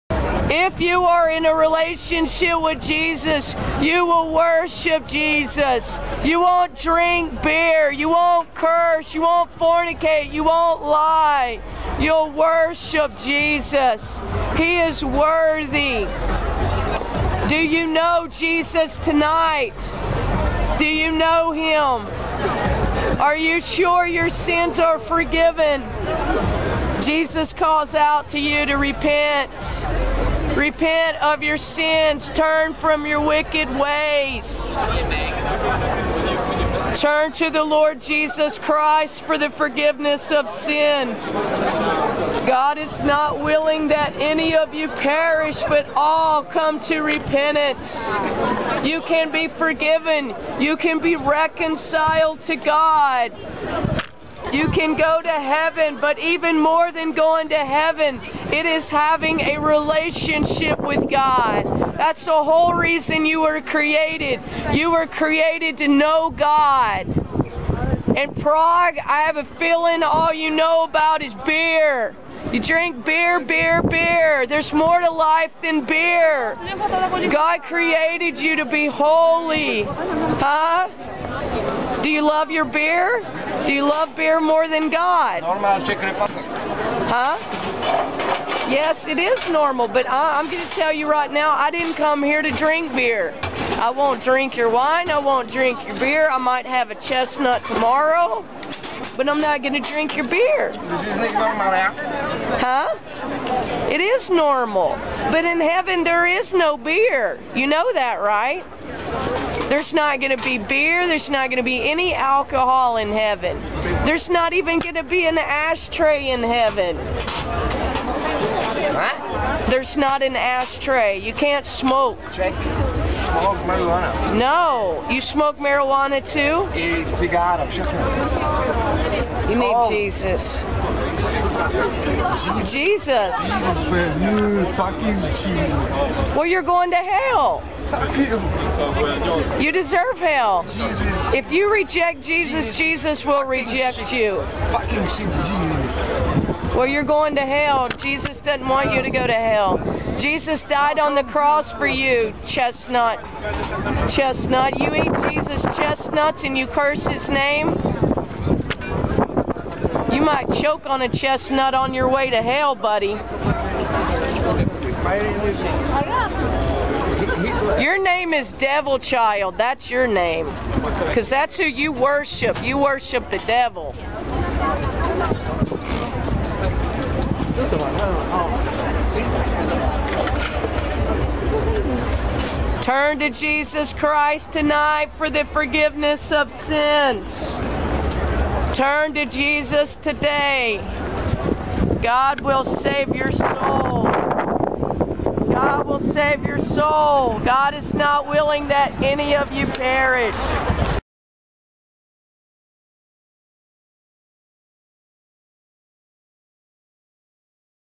Christmas Market Chestnut salesman / God Mocker headed for Hell
Chestnut guy didn't love Jesus and used God's name as a curse word. This man is headed for his chestnuts roasting on in a LAKE OF FIRE, unless he repents and comes to Jesus.